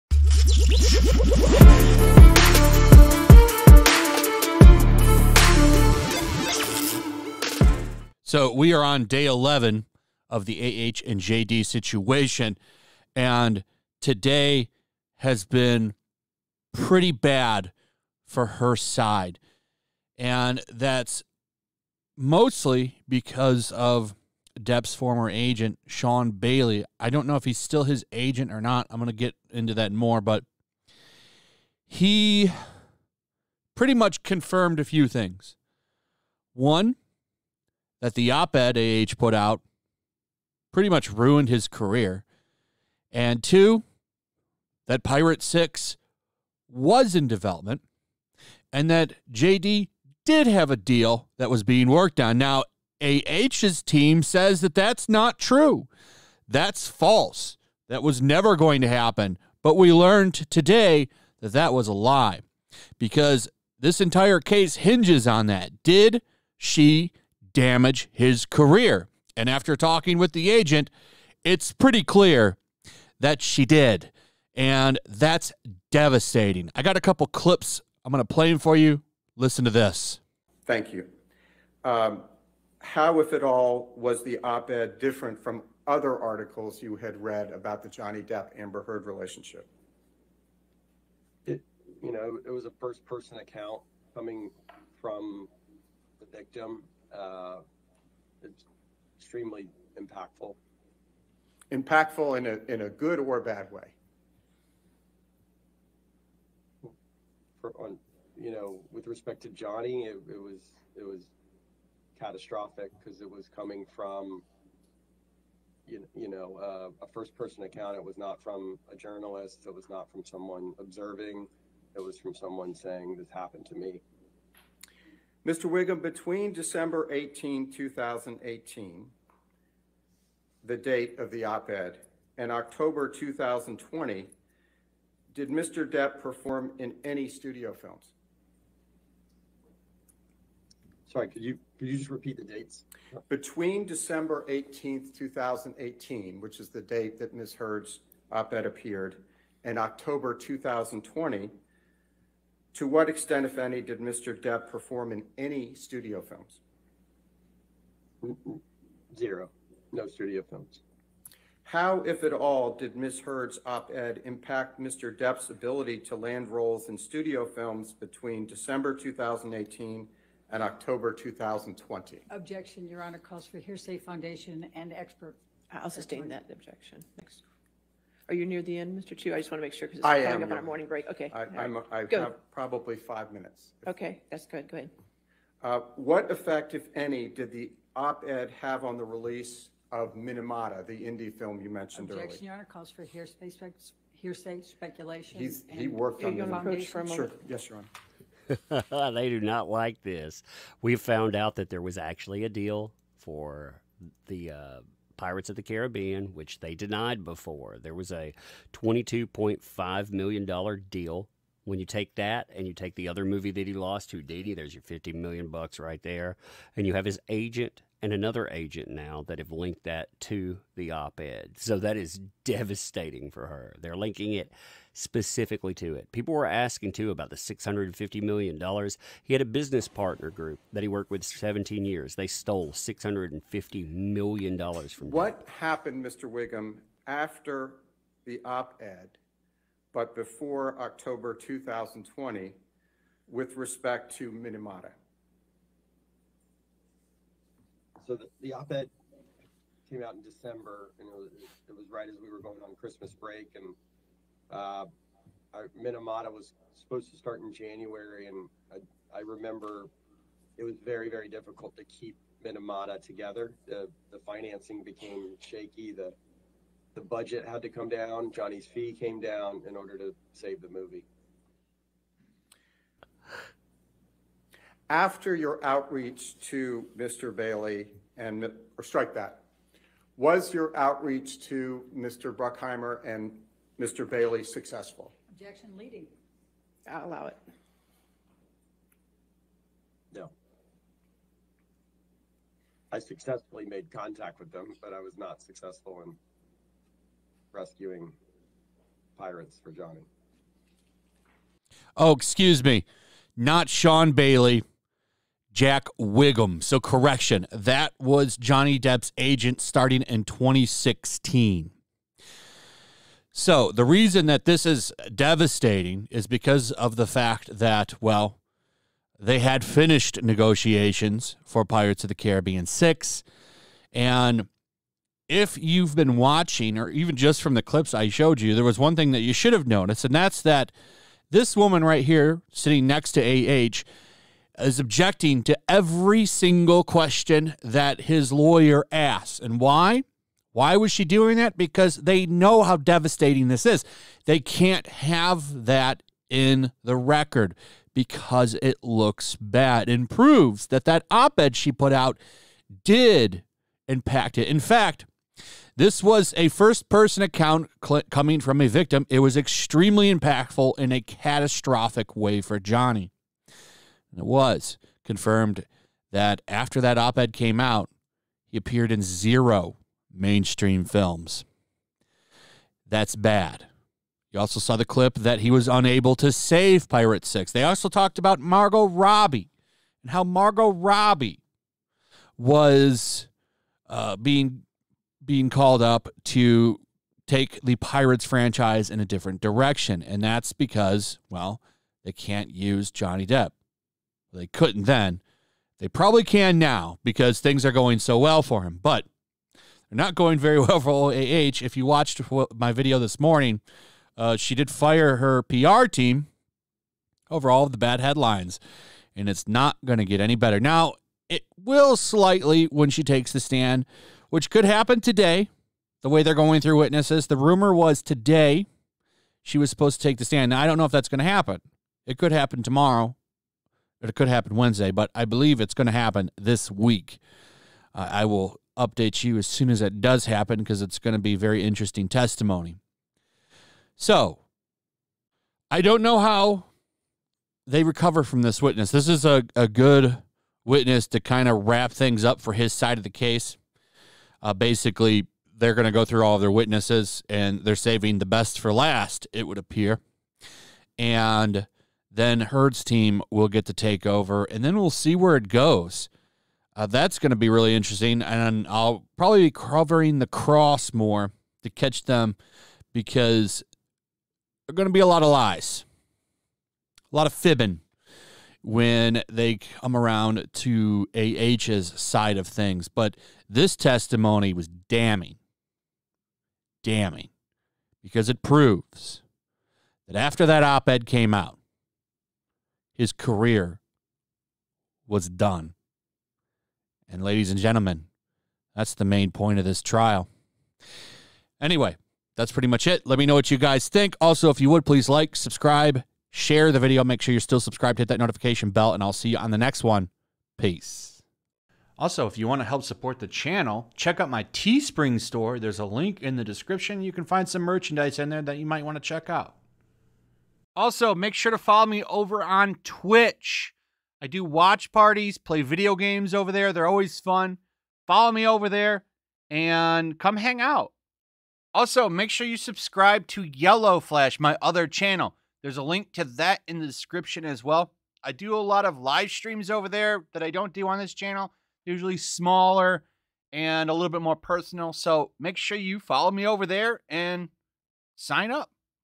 "It was CATASTROPHIC!" Hollywood agent CONFIRMS Heard's article DESTROYED Johnny Depp!
Johnny Depp vs. Amber Heard Trial - Day 12 is off to an explosive, and Johnny Depp's former Hollywood agent, a key witness confirms that his career was ruined by Heard's article!